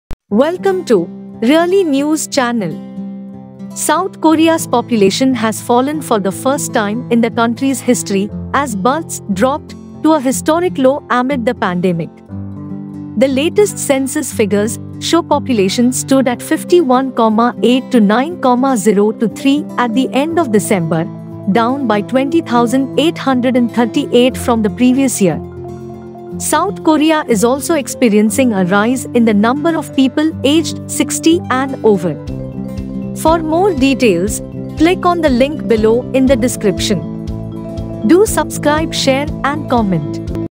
South Korea's population falls for first time in its history | Really News
South Korea's population has fallen for the first time in the country's history as births dropped to a historic low amid the pandemic. The latest census figures show population stood at 51,829,023 at the end of December, down by 20,838 from the previous year. South Korea is also experiencing a rise in the number of people aged 60 and over.